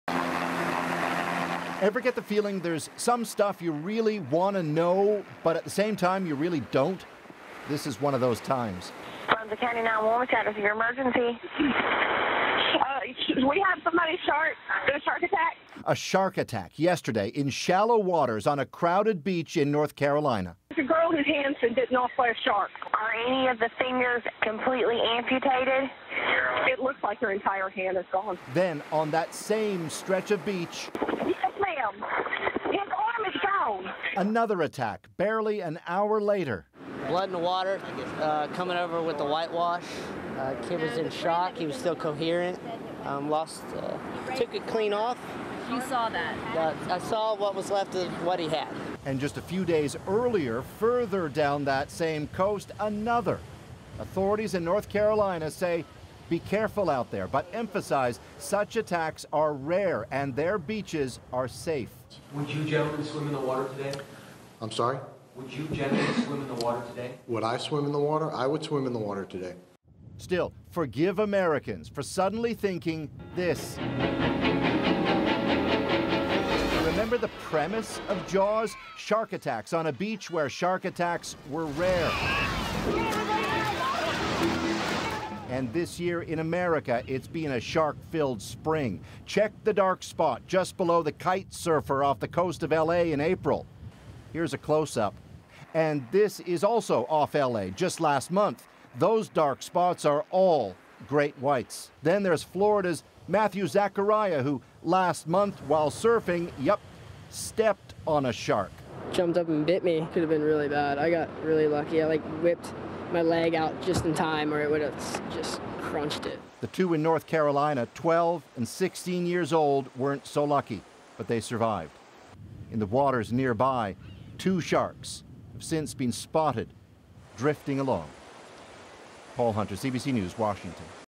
Shark attacks in North Carolina
Two young people were swimming in waist-deep water when they lost limbs in separate life-threatening shark attacks.